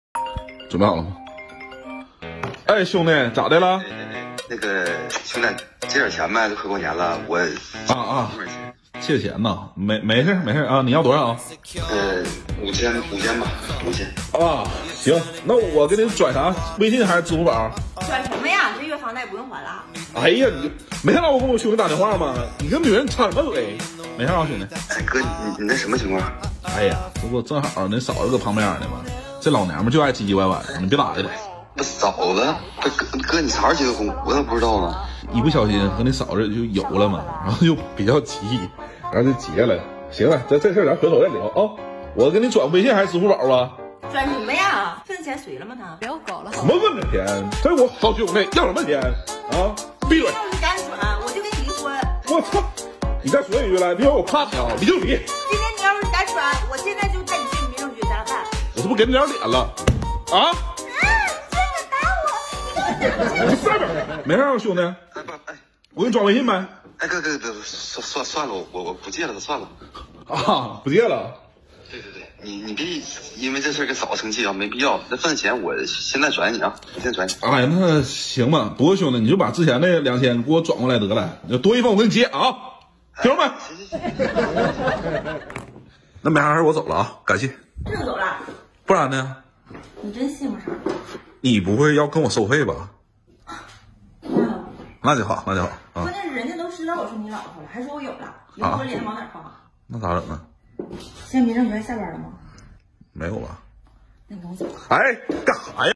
都知道我是你老婆了！这不合适啊，我不就找她帮了个忙么
性感黑丝老婆的日常幸福要求，老公直呼顶不住.